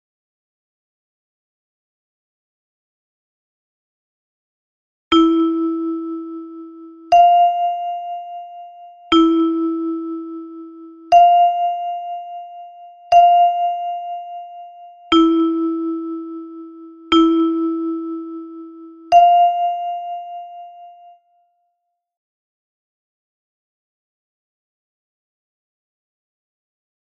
PET2 - ATD1 - primário
A linha mais baixa representa o lugar dos sons graves (som “grosso”), onde estão as bolinhas em vermelho. A linha mais alta representa o lugar dos sons agudos (som “fino”), onde estão as bolinhas em verde.